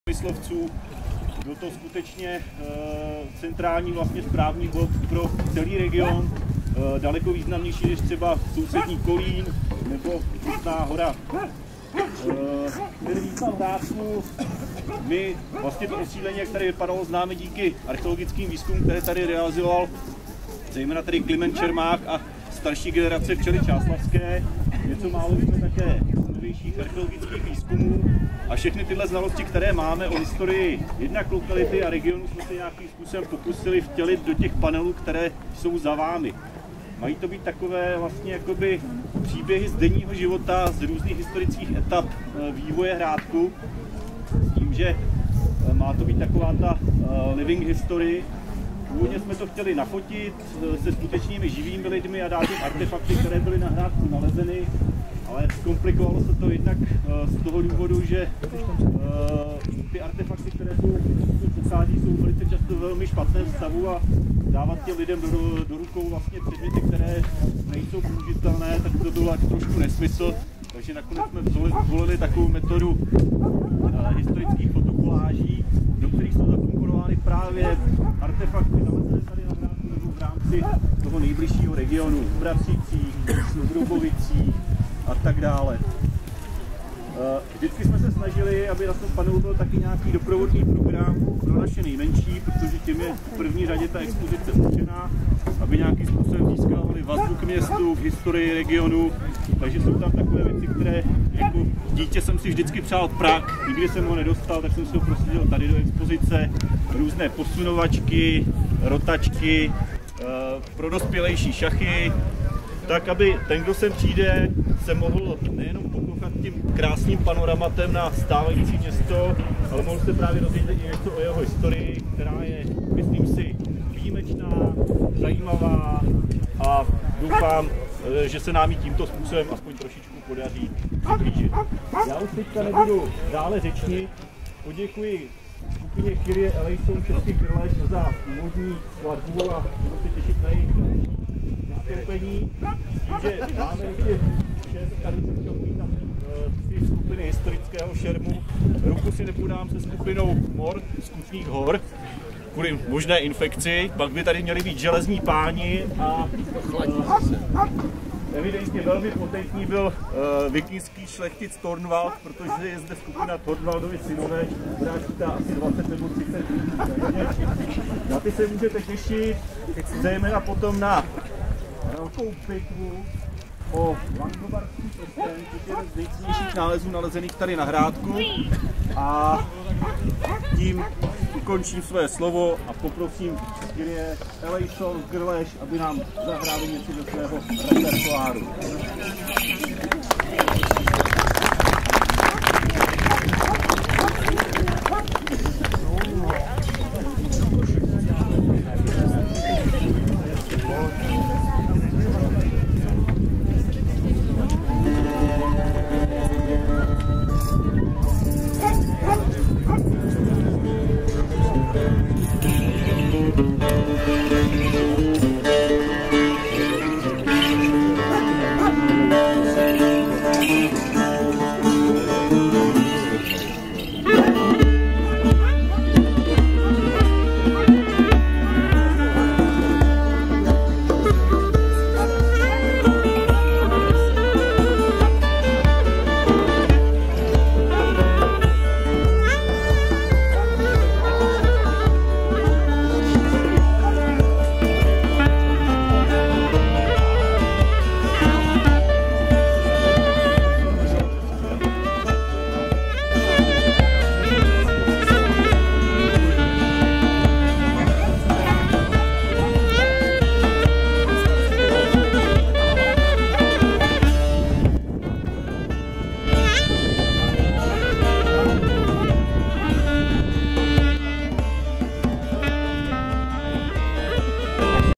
Otevření čáslavského Hrádku (mluví F. Velímský)
Čáslav - „Jarní rovnodennost na Hrádku,“ tak se jmenovala akce, při které byla slavnostně otevřena revitalizovaná archeologická lokalita Hrádek. Slavnost se konala v neděli 20. března odpoledne. Akci zorganizovalo Městské muzeum a knihovna Čáslav, Město Čáslav a Muzejní a vlastivědný spolek „Včela Čáslavská“.

Slavnost zahájil za Včelu magistr Jiří Žaloudek, oblečen byl v dobových šatech. Poté za město Čáslav promluvil doktor Vlastislav Málek, místostarosta města.

Jako třetí si vzal slovo doktor Filip Velímský z Archeologického ústavu Akademie věd Praha. Právě on totiž připravil projekt stálé venkovní informačně-edukativní expozice, sestávající z šesti panelů přibližujících dějiny této lokality. Informační tabule jsou interaktivní, na každé se mohou děti, ale i dospělí formou hry seznámit s historickými fakty daného místa. Umělecká a především vzdělávací hodnota panelů je na vysoké úrovni, je to cesta neformálního vzdělávání do budoucna.

Dále vystoupila hudební skupina dobové hudby Krless, šermířské skupiny Thorwald sons, Mor z Hor Kutných a Železní páni. Pro všechny účastníky bylo připraveno občerstvení a dobové soutěže pro děti. Na závěr se představili šermíři v boji o Langobardský prsten.

Čáslavský Hrádek v lesoparku Vodranty je místo s nejstarším osídlením města. Zde se nacházelo nejstarší opevněné místo na území dnešní Čáslavi. Bylo to původně hradiště, to bylo přestavěno na správní hrad, který však později zanikl. Dnes je toto místo zdrojem cenných archeologických nálezů. V posledních desetiletích ale zarostlo náletovými dřevinami. Tyto dřeviny se díky podpoře čáslavské radnice, dětí z místní základní školy, čáslavských judistů a především „Včely Čáslavské“ podařilo z velké části odstranit a místo opět zpřístupnit veřejnosti.

Poděkování  za zorganizování slavnostního otevření, potažmo za revitalizaci čáslavského Hrádku patří všem, kteří se těchto akcí zúčastnili. A co popřát Hrádku? Aby započatá revitalizace byla dotažena do zdárného konce a aby se podařilo lokalitu v dobrém stavu udržet i v budoucnosti.